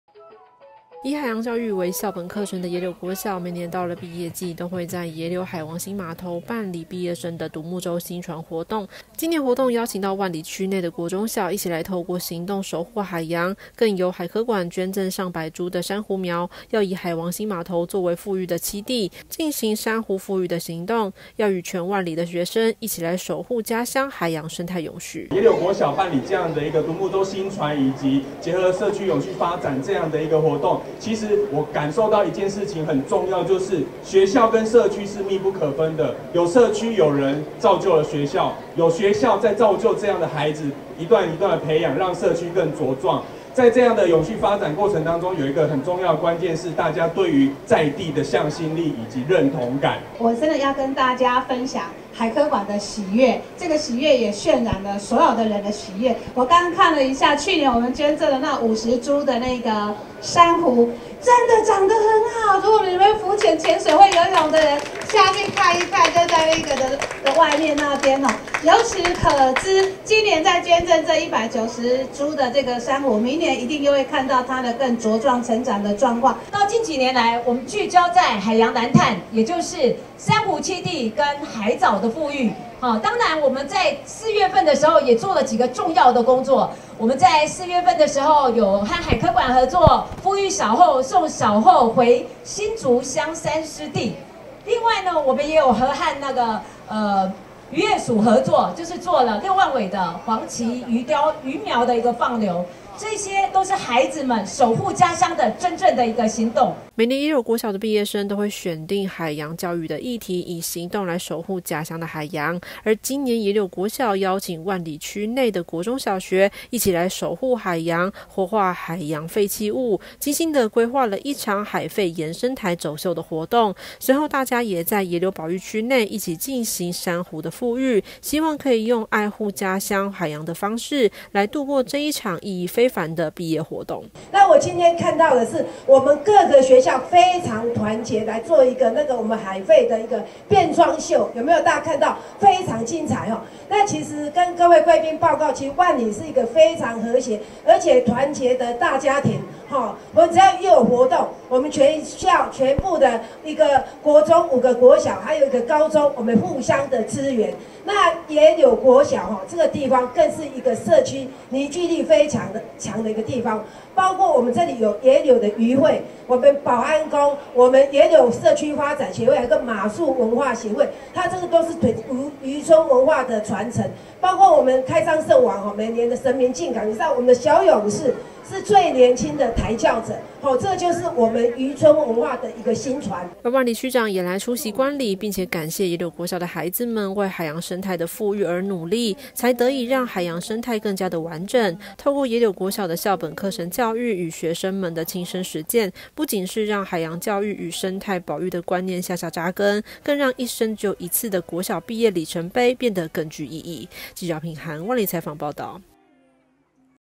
野柳國小畢業活動 邀萬里國中小復育珊瑚
以海洋教育為校本課程的野柳國小在野柳海王星碼頭辦理畢業獨木舟薪傳活動，今年除了薪火傳承，更由海科館捐贈上百株的珊瑚苗，以海王星碼頭作為復育棲地，進行珊瑚復育行動，野柳國小更邀請萬里區的國中、小一起來共襄盛舉，讓全萬里的學生與野柳國小畢業生一起透過行動來守護家鄉海洋，讓生態永續。